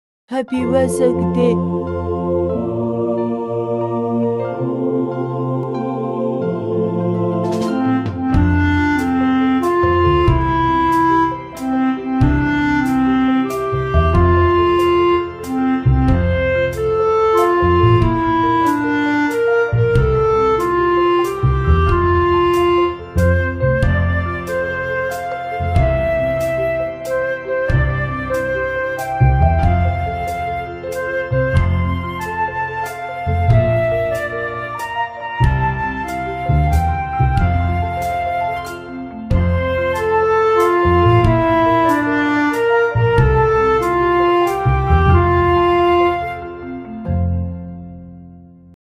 Happy Vesak Day
Every year on the 15th day of the lunar calendar, the United Nations Vesak Day celebrates the birth, enlightenment and Nirvana of the Buddha.